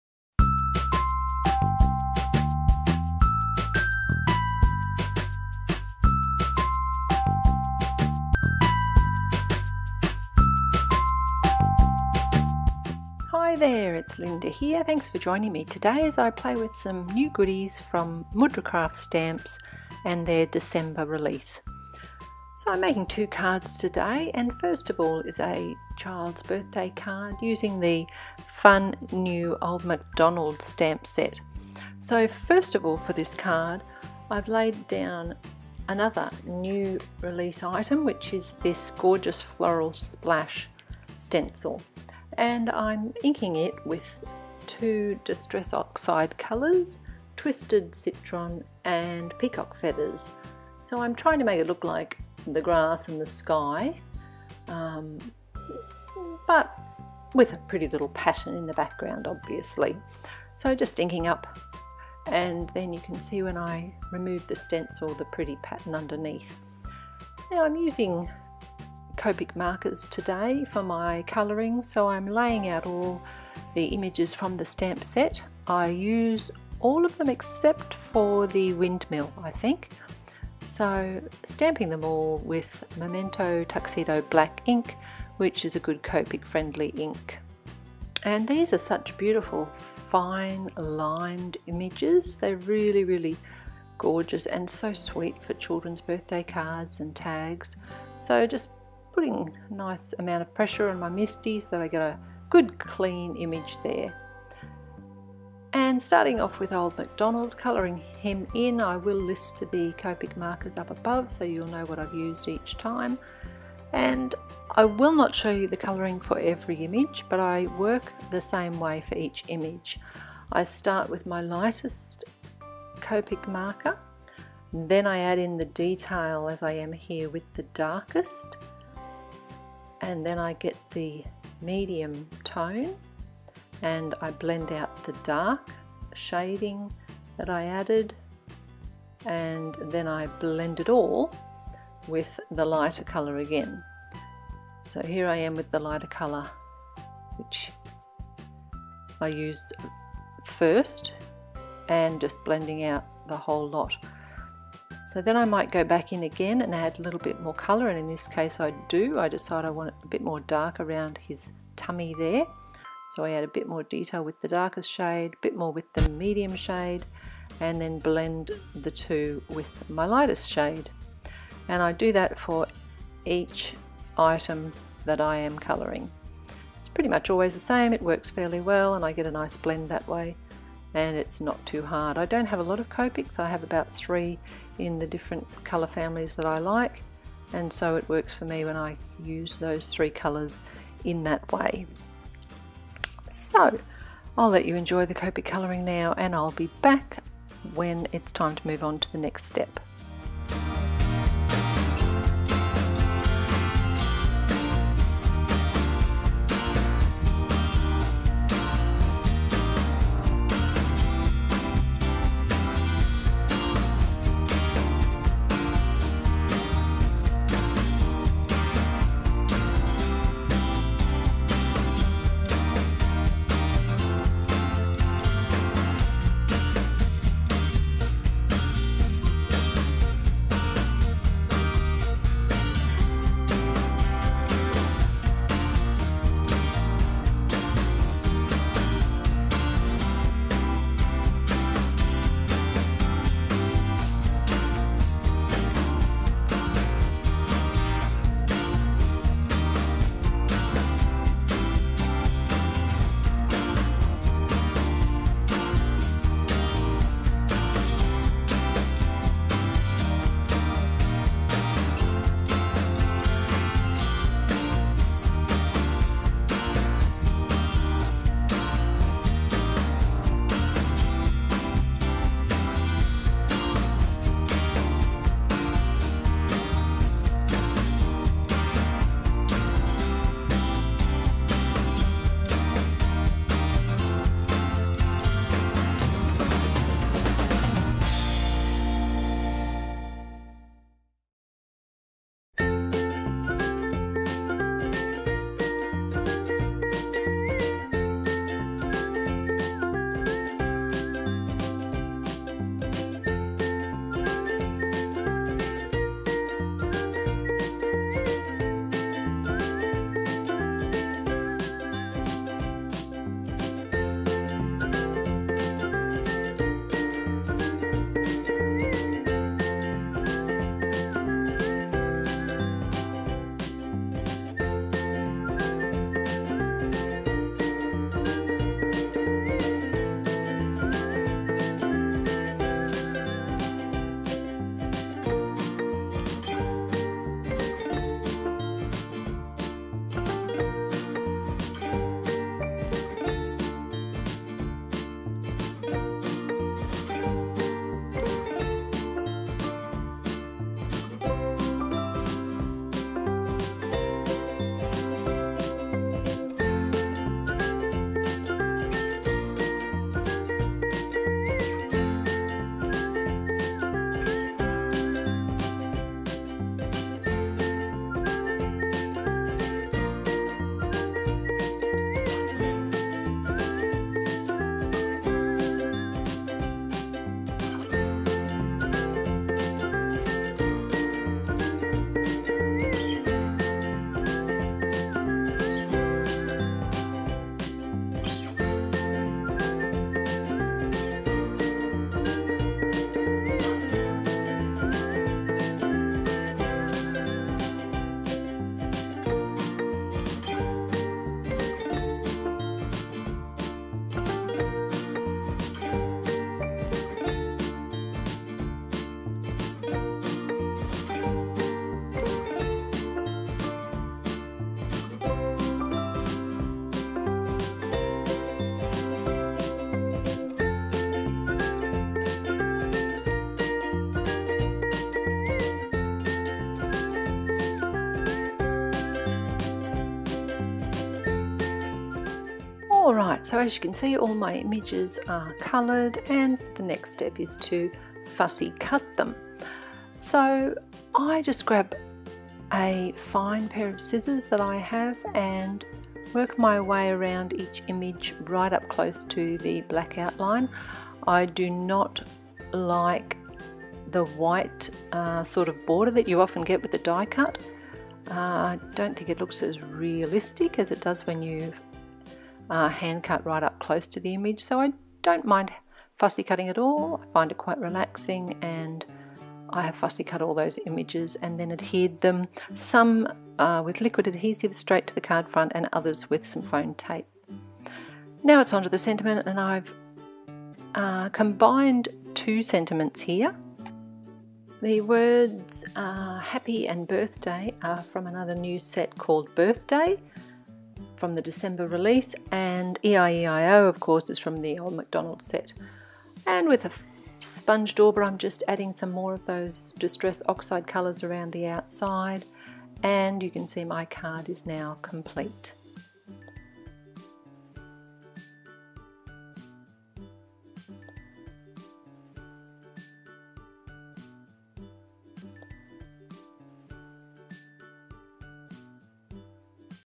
Old MacDonald Card
Hi, it's Linda here - thanks for joining me today:) You will see how to make a fun kid's card with the adorable images in the Old Macdonald stamp set.